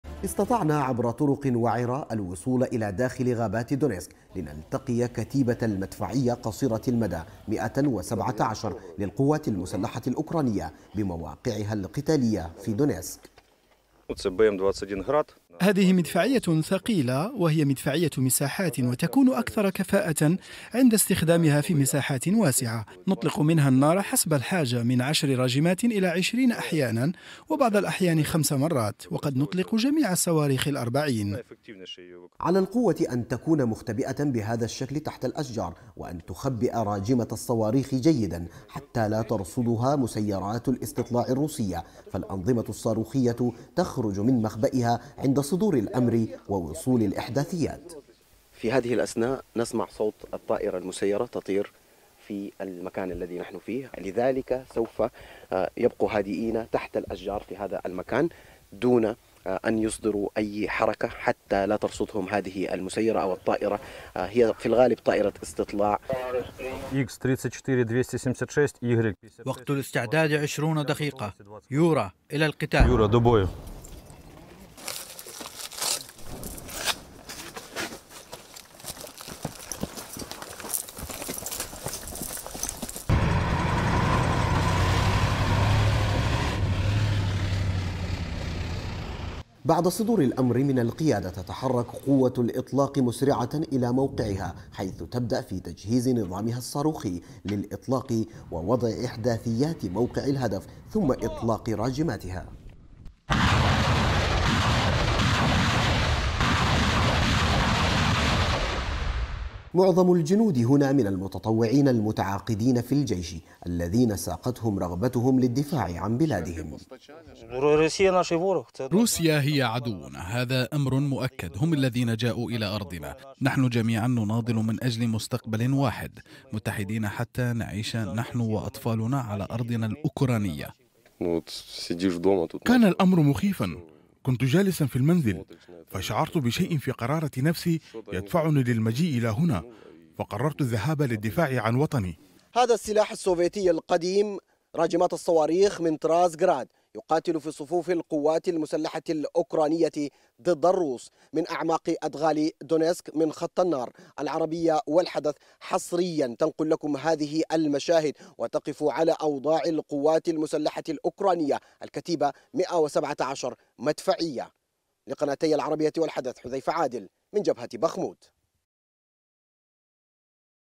العربية ترصد القتال في أدغال غابات دونيتسك
العربية تصل إلى أعماق أدغال دونيتسك وترصد هجوم الكتيبة 117 الأوكرانية على القوات الروسية براجمات الصواريخ
لمعرفة أحدث المستجدات.. عبر البث المباشر من هنا ⤵
وبإمكانكم متابعة العربية على منصات التواصل